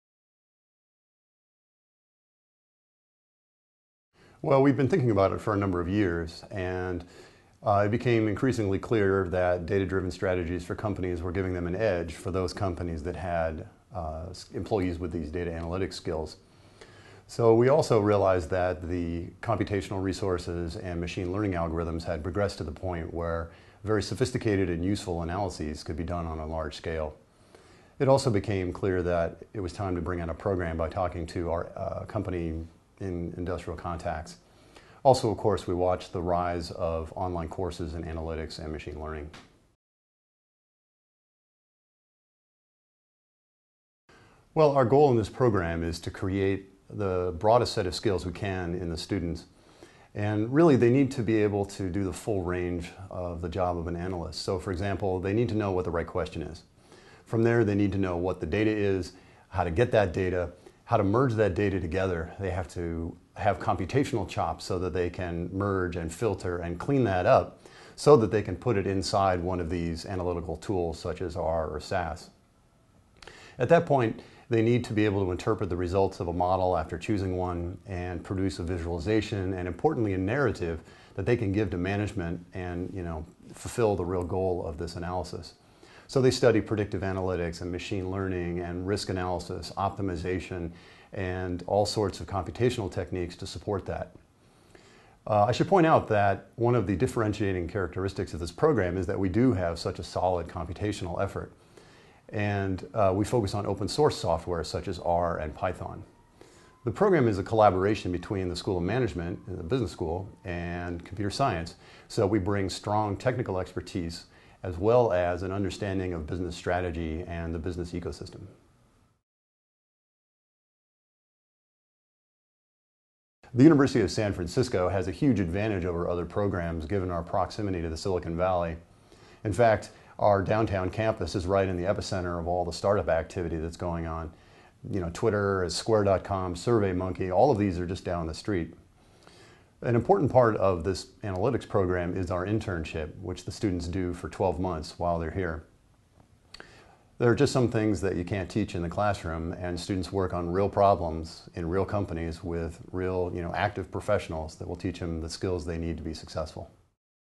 Big Data: Terence Parr
Terence Parr, an associate professor of computer science at the University of San Francisco, discusses the school's new Master of Science in Analytics program. (From Campus Technology February 2013. Video courtesy of University of San Francisco)